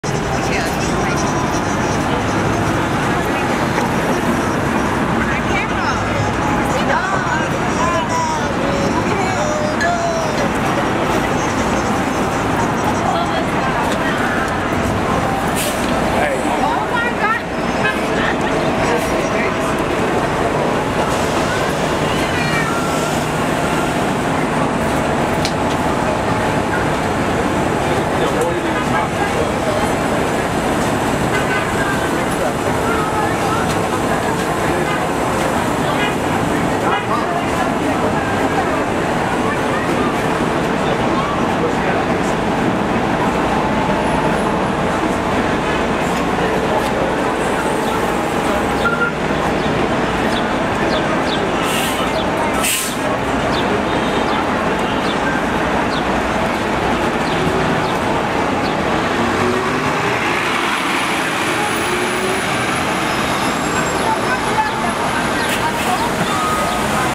MTA New York City Bus # 5469 on the Bx12 +Select Bus Service+
MTA New York City Bus: 2018 NovaBus LFSA # 5469 on the Bx12 +Select Bus Service+ to Bay Plaza/Co-Op City approaches, alights, and departs the bus stop at Fordham Road and 3rd Avenue.